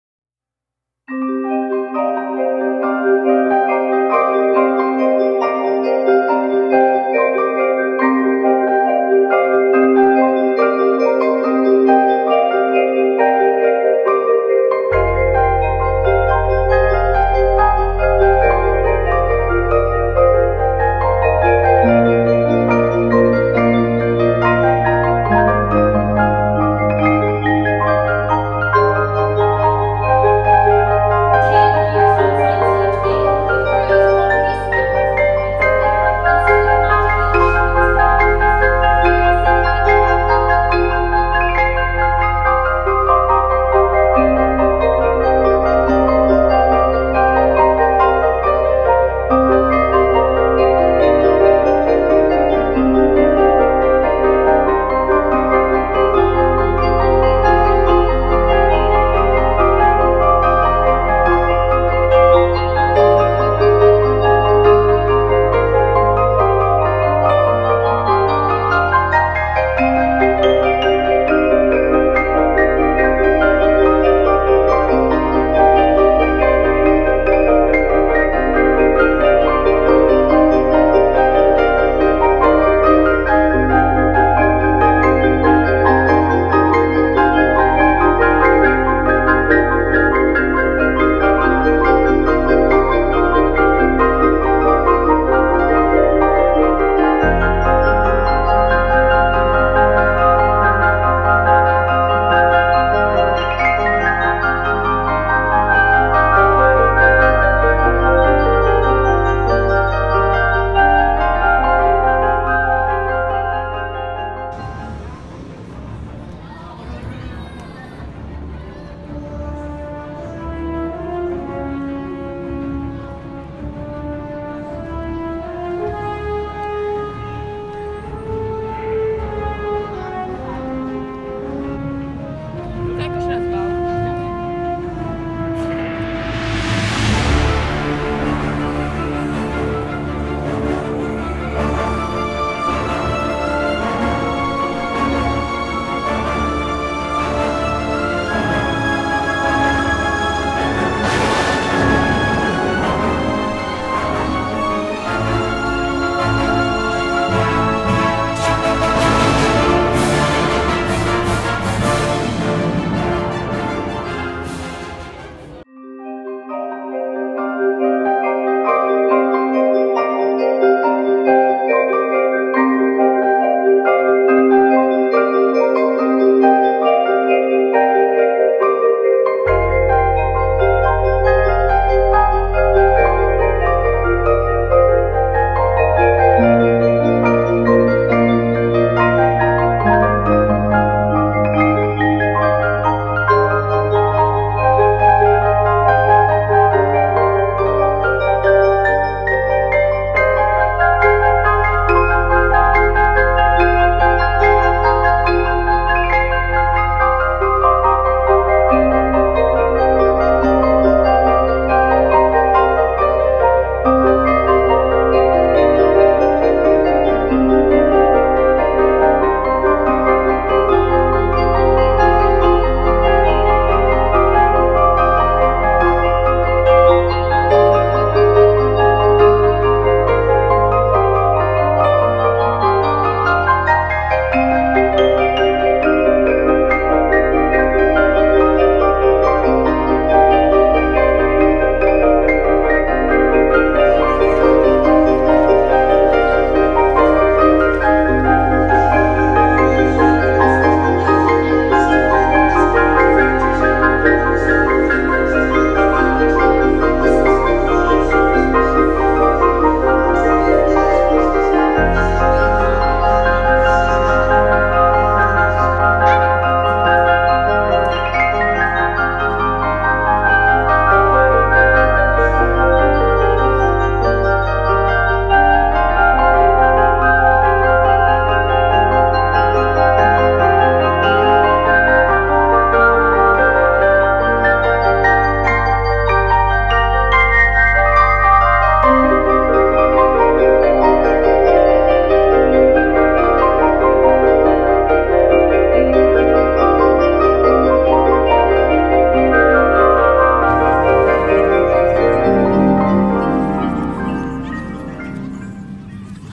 Warner Brothers Harry Potter Studio Tour |London Travels
This is a terribly late - 1 year late- video upload from my trip to London. People usually spend about 4 hours inside and you'll be surprised at how long you'll spend marvelling at the set there! Heres a sneak peak ;)